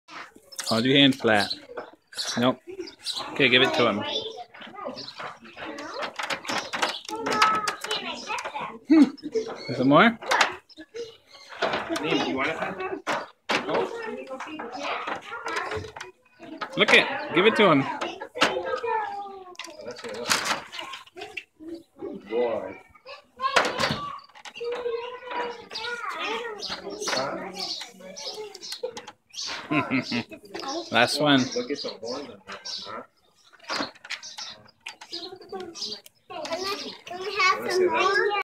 Alexis feeding some goats at Forest Park Zoo
Alexis feeds some goats at Forest Park Zoo